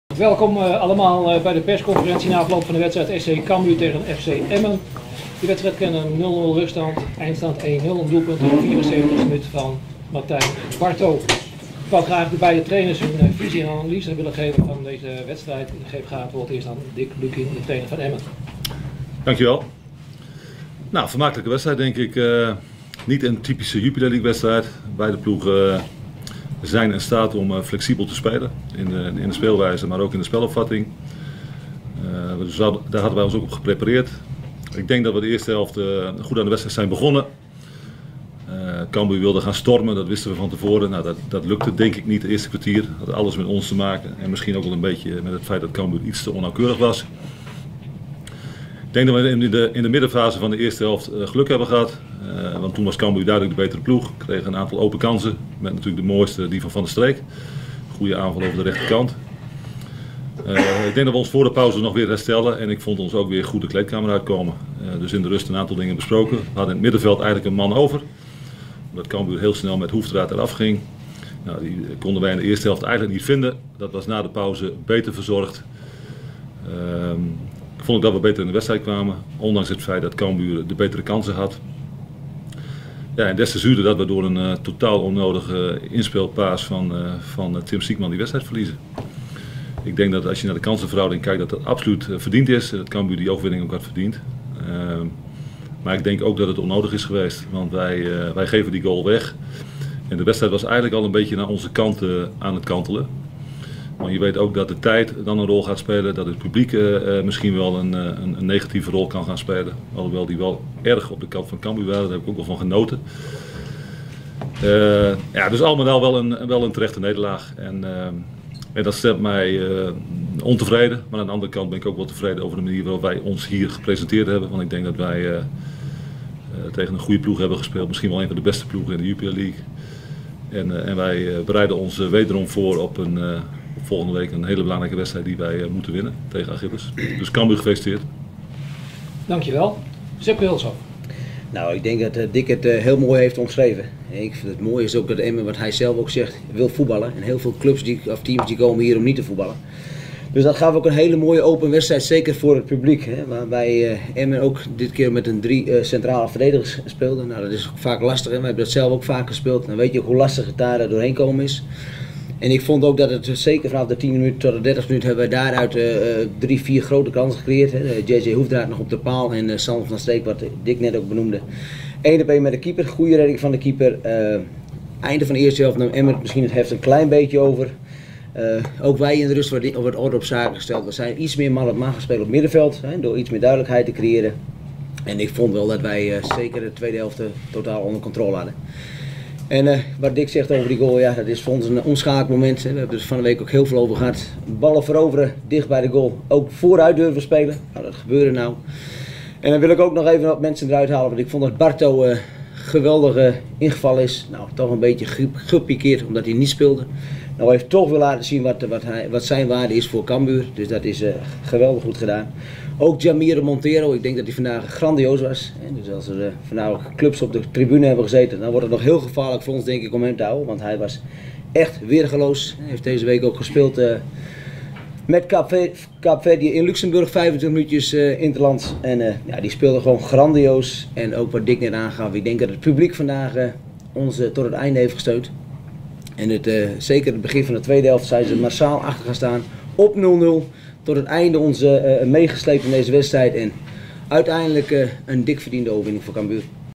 Persconferentie SC Cambuur - FC Emmen
Persconferentie na afloop van het duel SC Cambuur - FC Emmen.

Productie: Omroep Leo